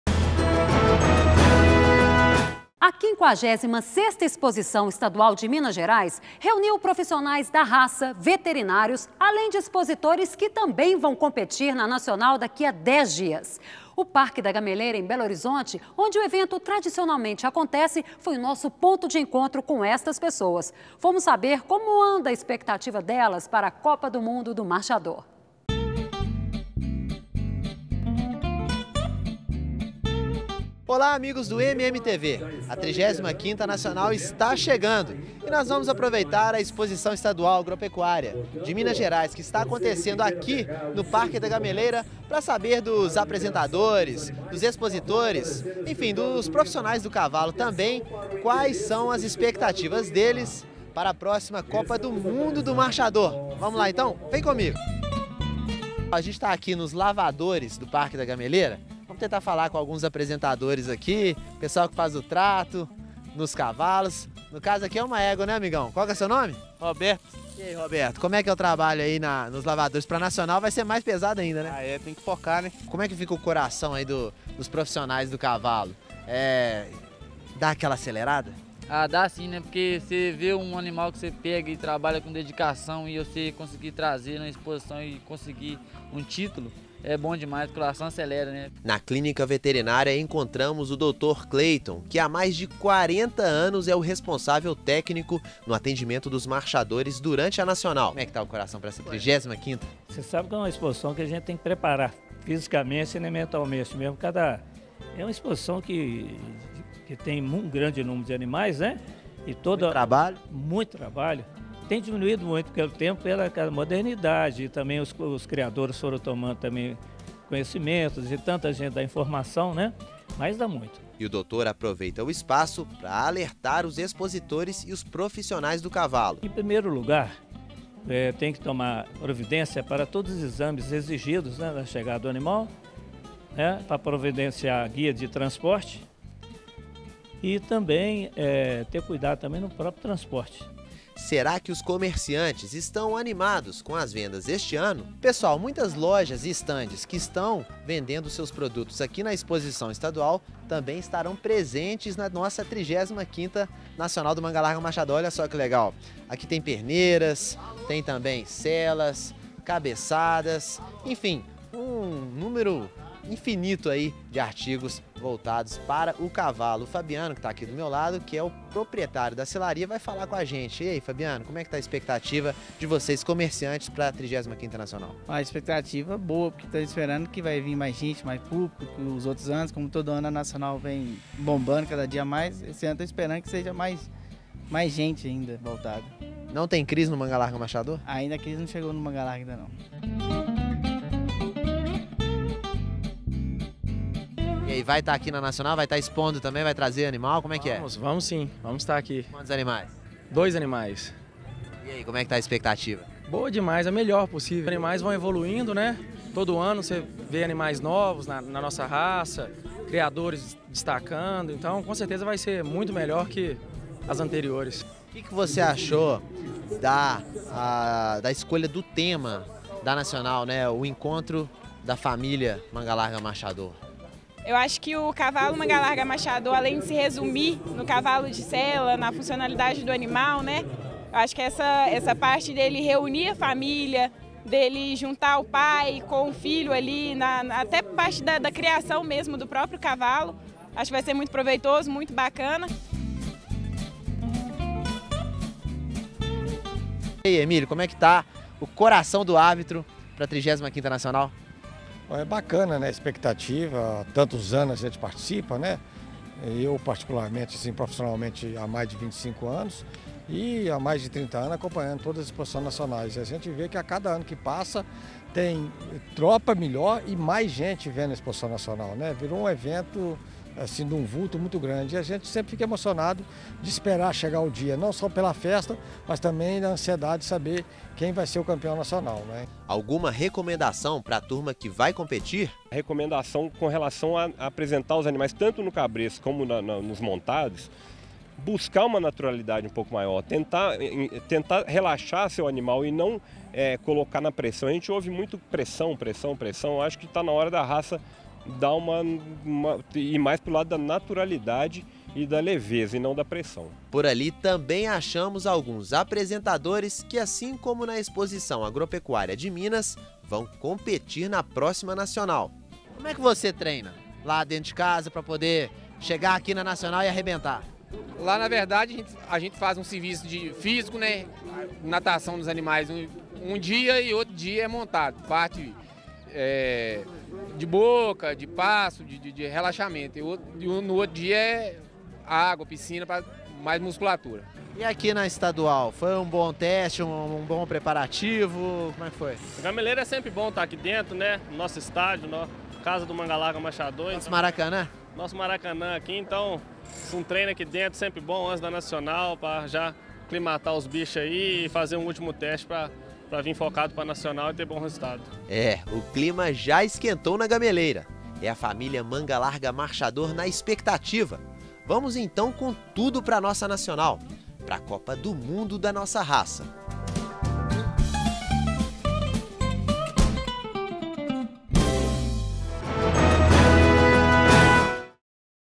Expectativa Nacional 2016 (35ª Nacional)
A Quinquagésima Sexta Exposição Estadual de Minas Gerais reuniu profissionais da raça, veterinários, além de expositores que também vão competir na Nacional daqui a dez dias. O Parque da Gameleira, em Belo Horizonte, onde o evento tradicionalmente acontece, foi o nosso ponto de encontro com estas pessoas. Fomos saber como anda a expectativa delas para a Copa do Mundo do Marchador.